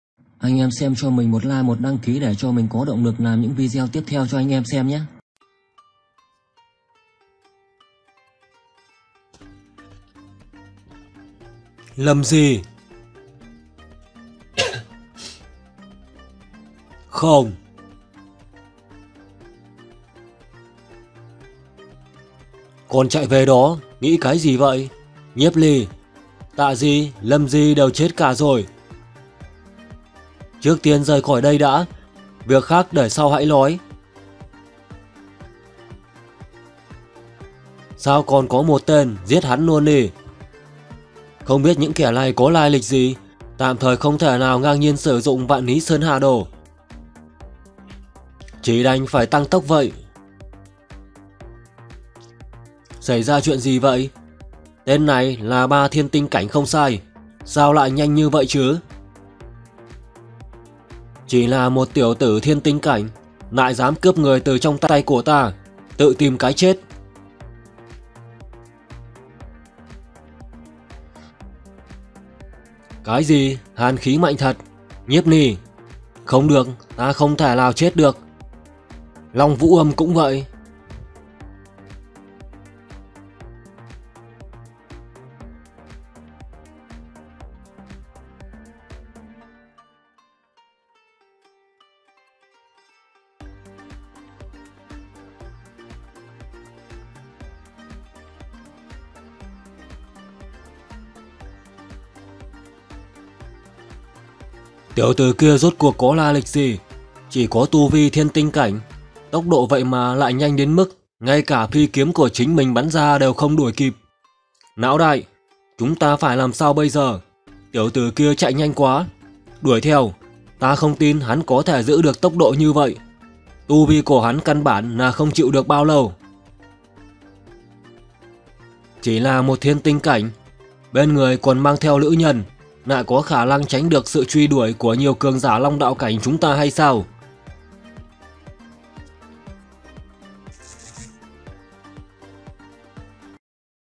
Yêu Thần Ký Tập 486|Yêu linh sư mạnh nhất đại lục Nhiếp Li
Yêu Thần Vừa Xuất, Ai Dám Tranh Phong.Yêu Linh Sư Mạnh Nhất Thánh Linh Đại Lục Nhiếp Li, bởi vì một cuốn sách thần bí Yêu Linh Thời Không mà trở về năm 13 tuổi, tu luyện mạnh nhất công pháp, mạnh nhất yêu linh chi lực, đột phá tới võ đạo đỉnh phong...Kẻ thù kiếp trước, toàn bộ thanh toán...Nếu như đã trùng sinh, thì kiếp này ta là chúa tể đại lục, Vạn Thần chi Vương,để cho tất cả run rẩy dưới chân ta đi.
►►►►►► Nguồn Truyện: Vcomi.me  - fuhuz.com - Manhuarock.net - Toptruyen.net - Truyensieuhay.com  - Vcomic.net